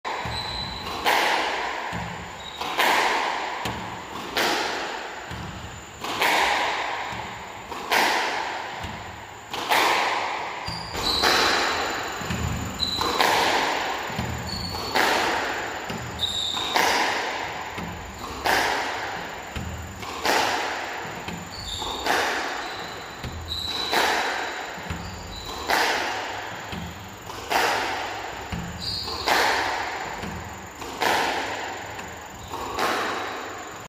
Mansoor Zaman 🇵🇰 Givings tips how to improve backhand and forehand drive in Squash.
*To improve your forehand and backhand drives in squash, here are some key tips and techniques

*Focus on Accuracy and Control: While power is important, accuracy and control are equally essential. Aim to hit the ball consistently to specific targets on the front wall to improve your shot placement.

*Incorporate Drills: Engage in various drills that specifically target forehand and backhand drives. These drills can involve hitting against the front wall, practicing cross-court shots, or incorporating movement patterns into your training.

*Remember, consistent practice, patience, and attention to detail are key to improving your forehand and backhand drives in squash.